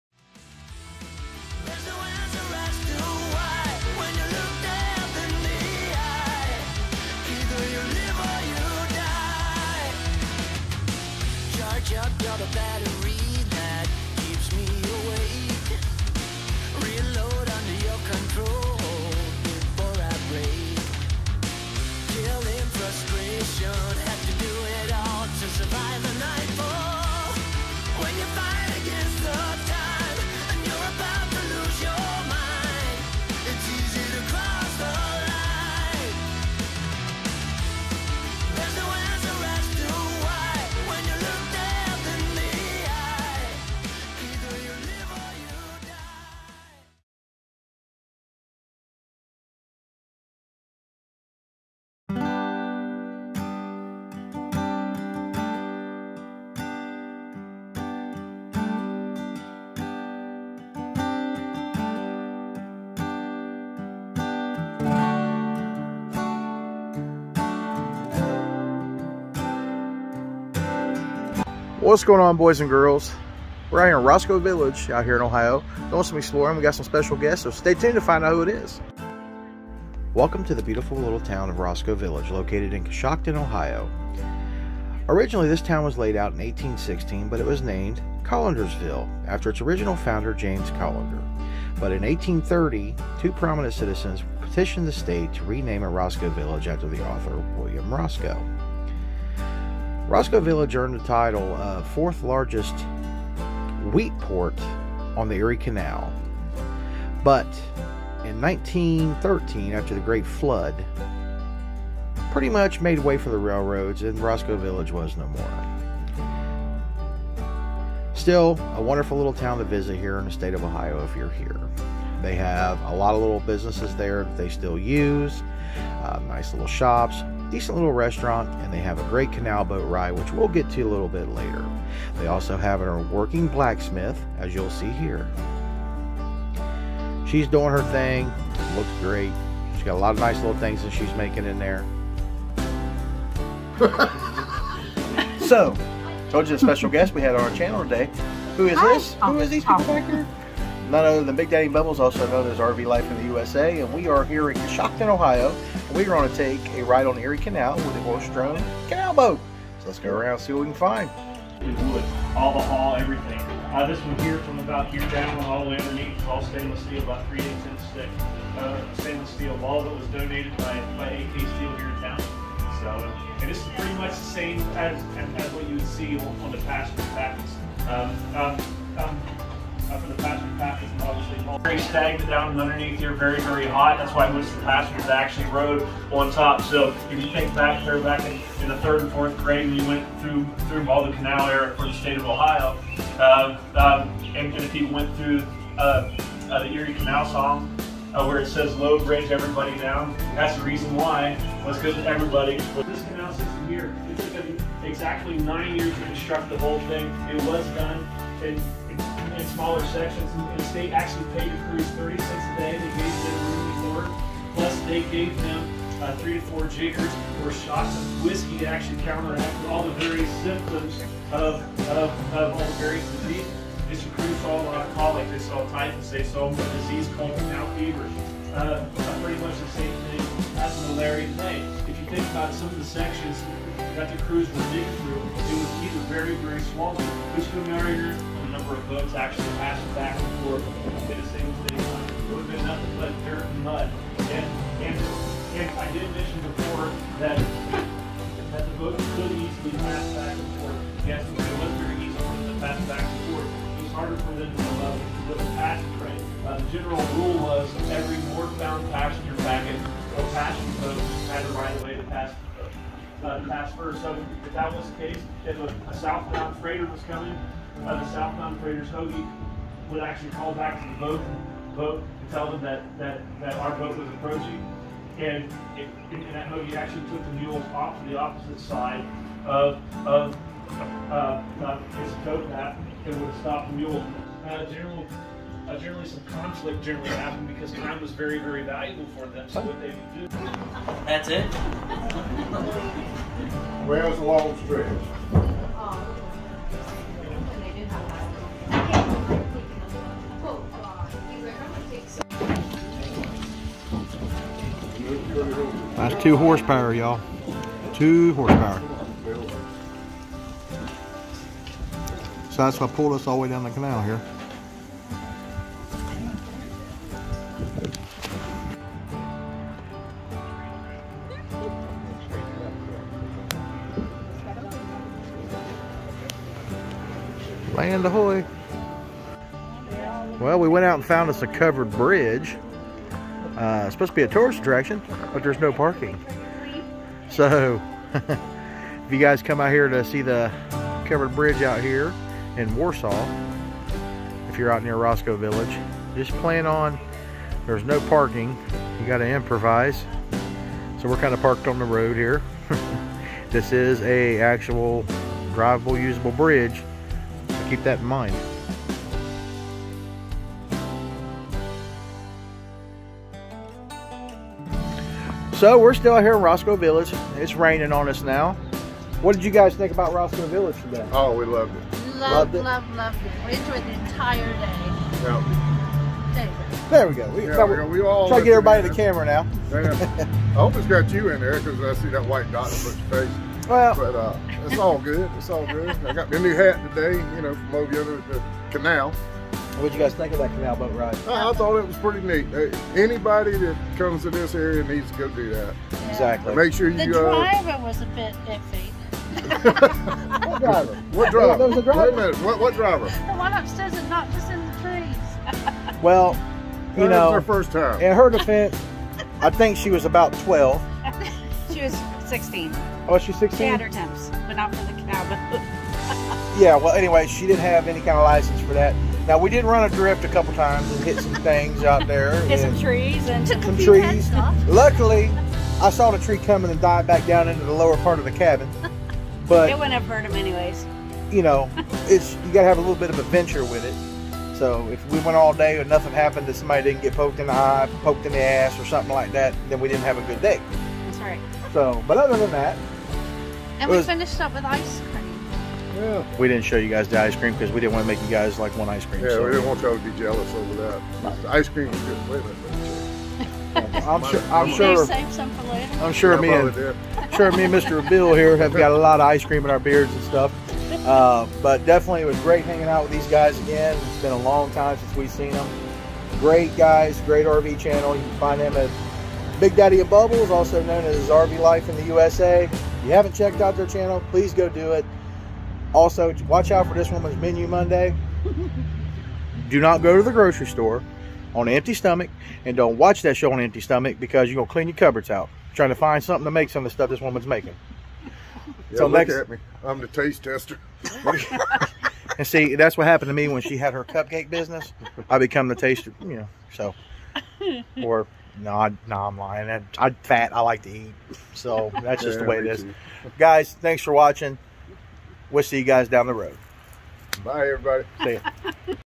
ROSCOE VILLAGE OHIO ERIE CANAL//RV LIVING FULLTIME
come join us on our visit to roscoe village and step back in time with a ride on a horse driven canal boat, with special guests RV LIFE IN THE USA aka big daddy and bubbles on YouTube